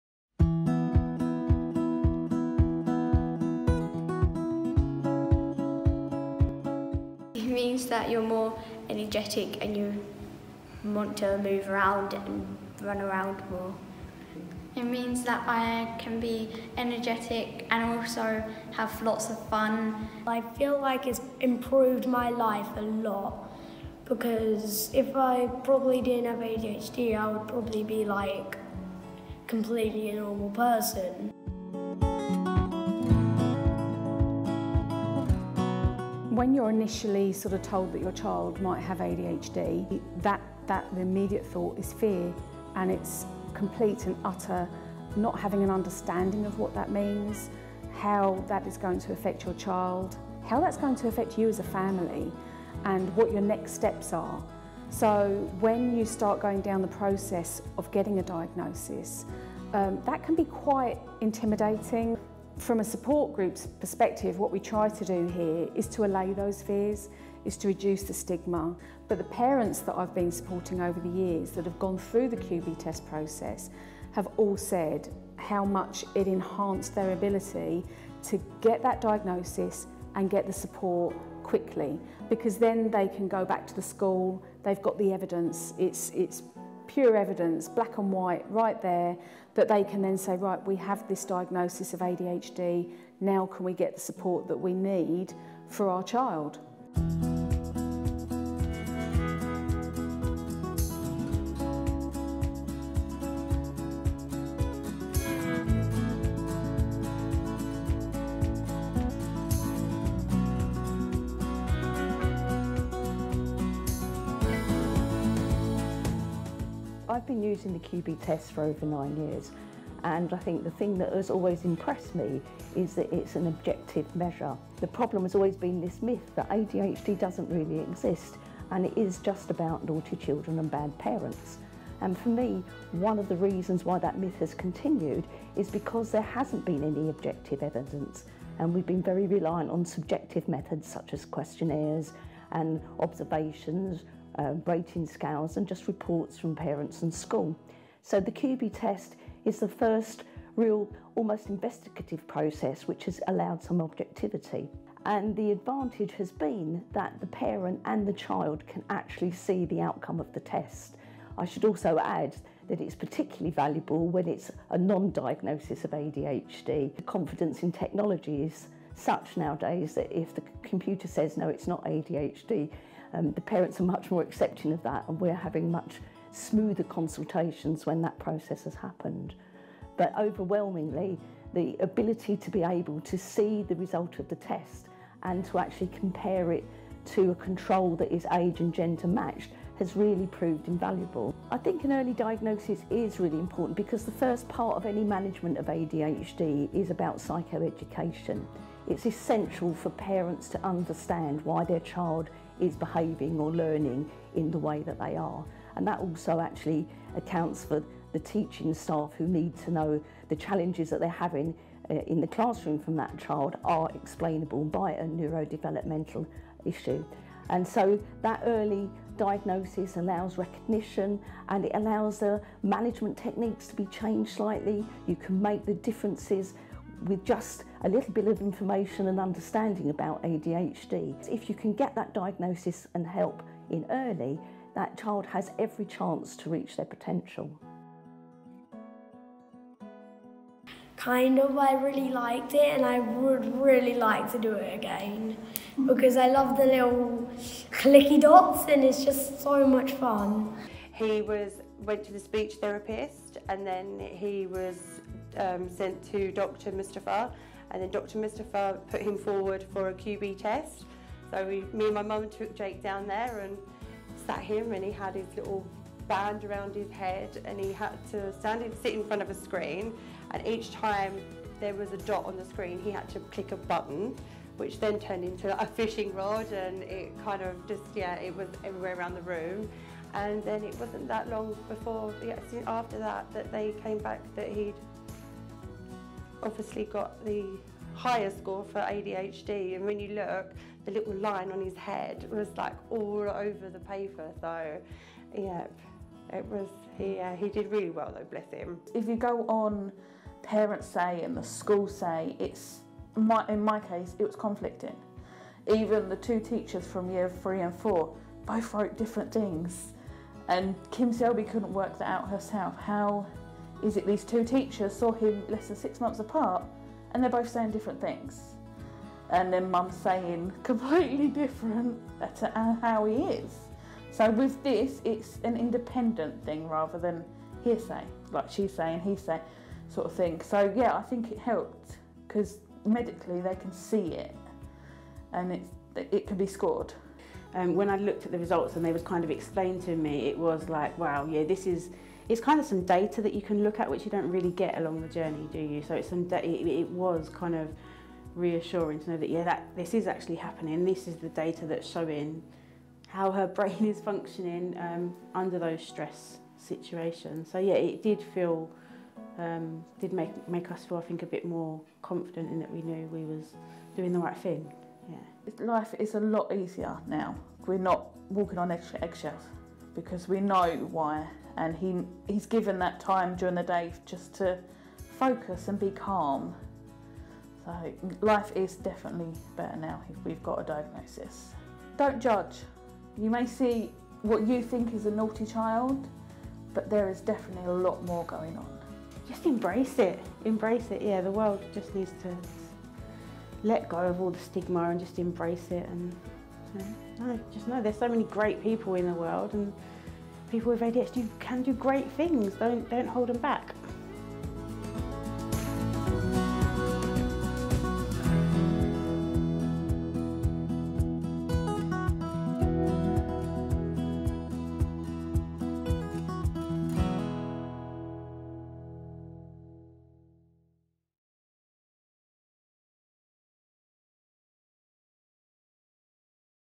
Living with ADHD and the importance of early diagnosis
The experience of living with ADHD and the importance of a confirmed diagnosis as told by young people, parents and healthcare professionals in Medway.

AHSNs are working with mental health trusts and community paediatric services to improve the assessment process for Attention Deficit Hyperactivity Disorder (ADHD) using computer-based tests (measuring attention, impulsivity and activity). AHSNs, including KSS AHSN, are supporting its wider spread during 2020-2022 as part of an AHSN Network national programme.

We are supporting an ADHD intervention which uses technology created by health technology company Qbtech Ltd. The technology, QbTest, measures a patient’s attention, impulsivity and motor activity all at the same time. These indicators are core symptoms of ADHD and accurate measurement adds objectivity to support timely diagnosis.

The intervention has shown a reduction in time to diagnosis by 153 days (median). Using this intervention increases staff capacity by reducing the number of appointments needed for diagnosis and by ruling out ADHD sooner for ambiguous cases. Releasing capacity of clinicians can help reduce waiting lists and allow clinicians more time to concentrate on complex cases.

Published November 2021.